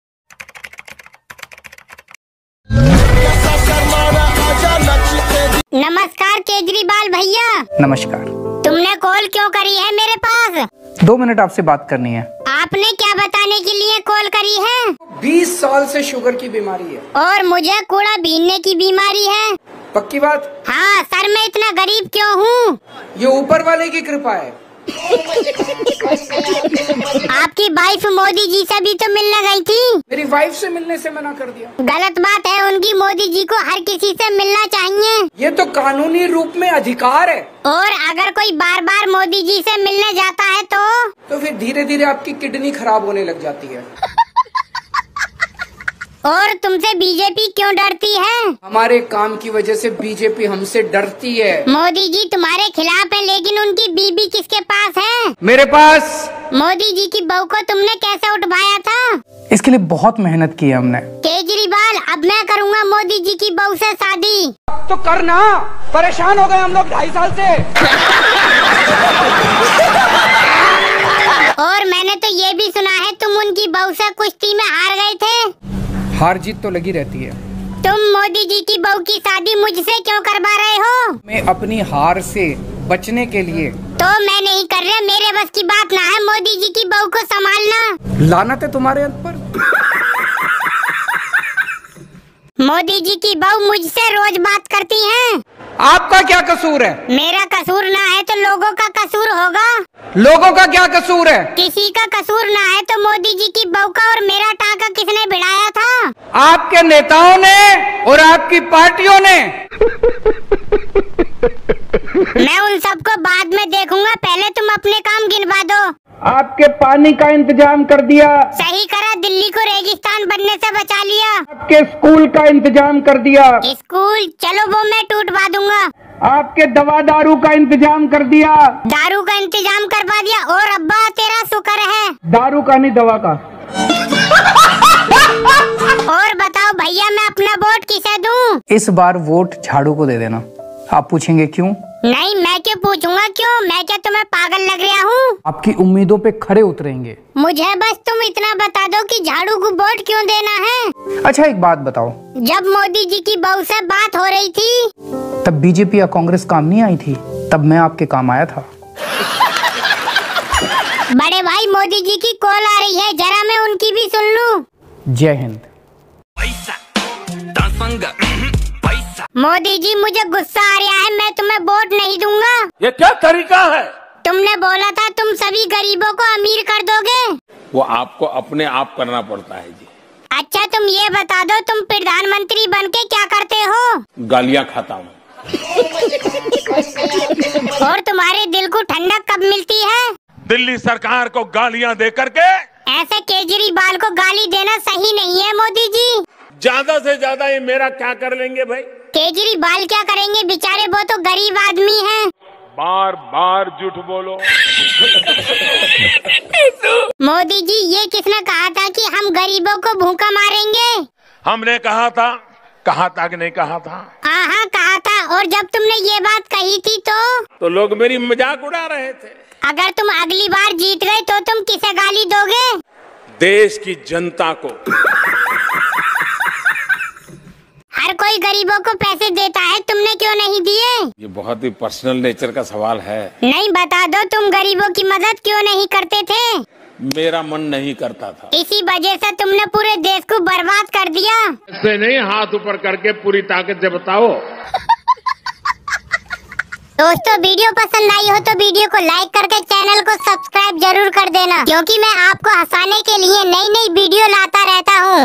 Arvind Kejriwal vs Billu vs Modi | Narendra Modi funny call | Arvind Kejriwal comedy | Billu Raja
चेतावनी }
इस वीडियो का मकसद किसी की भावनाओं को ठेस पहुंचाना या इंसर्ट करना नहीं है इस वीडियो का मकसद आप का मनोरंजन करना है हम सभी कलाकारों का सम्मान करते हैं |